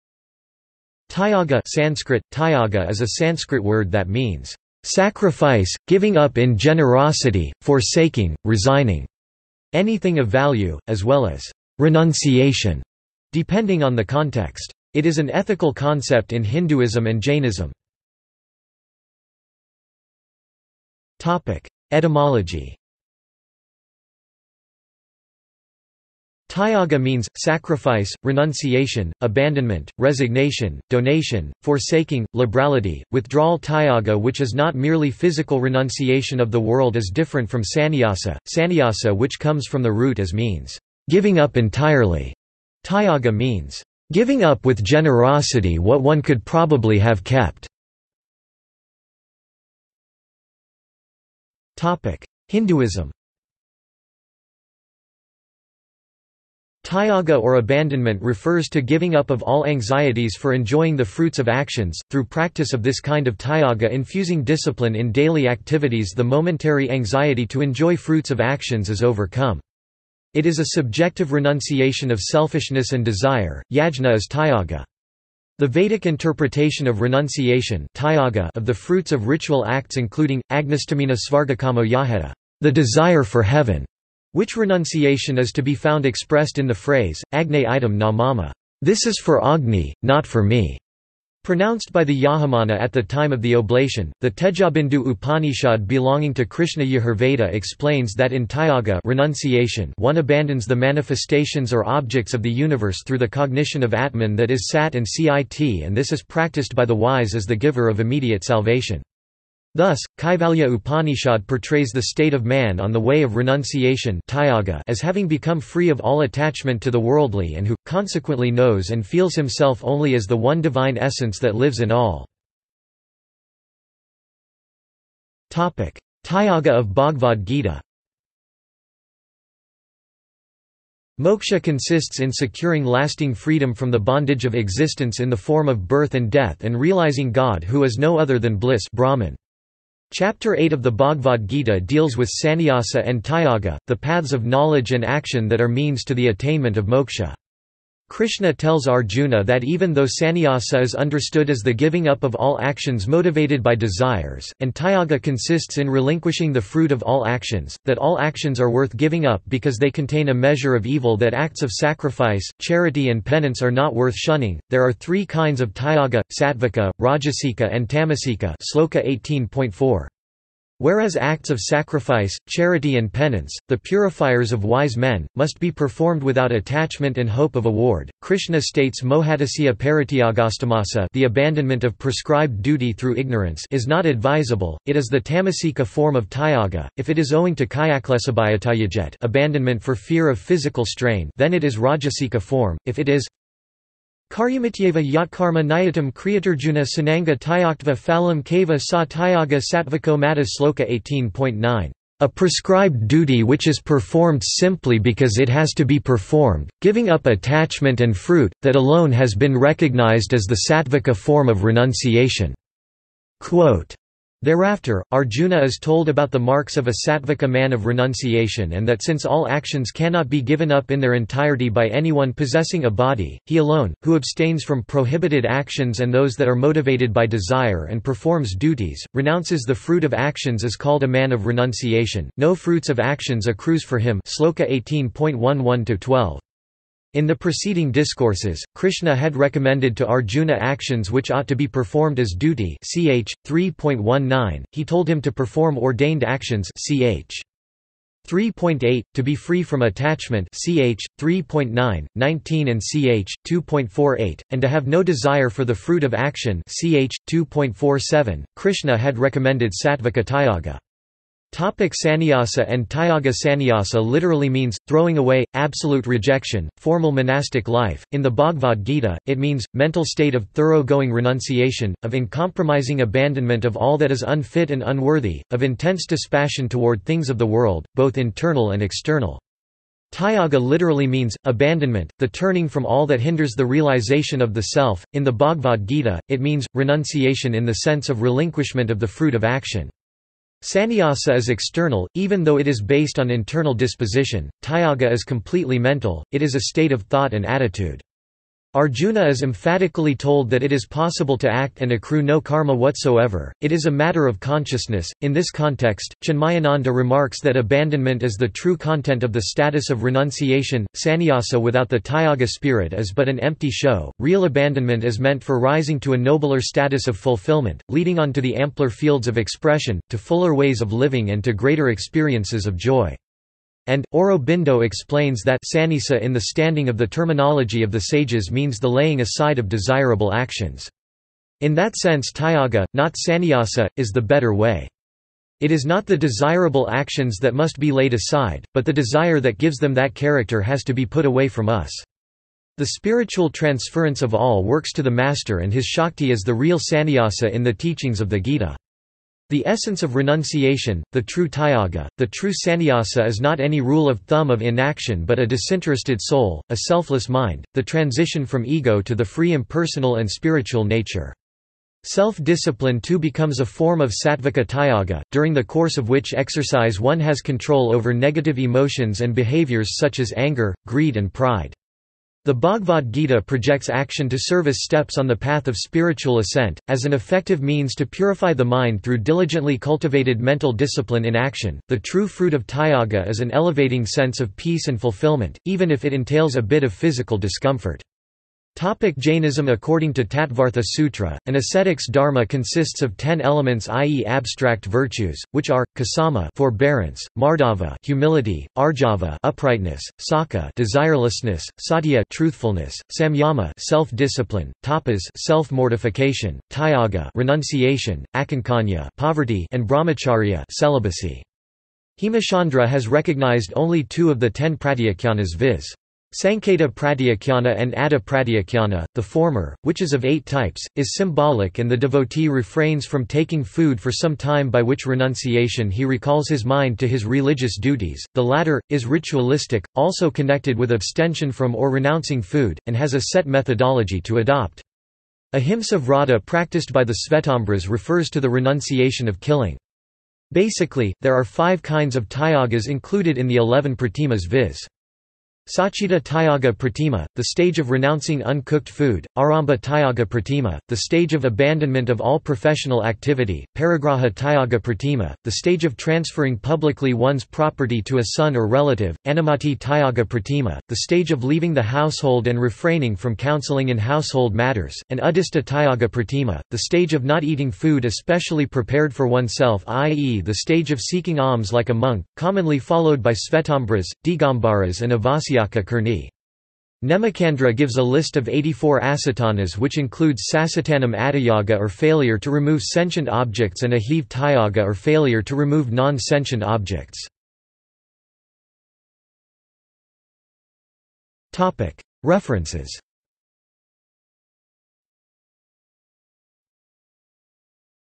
Tyāga | Wikipedia audio article
This is an audio version of the Wikipedia Article:
Tyāga

- Socrates

SUMMARY
Tyāga (Sanskrit:  त्याग) is a Sanskrit word that means "sacrifice, giving up in generosity, forsaking, resigning" anything of value, as well as "renunciation" depending on the context. It is an ethical concept in Hinduism and Jainism.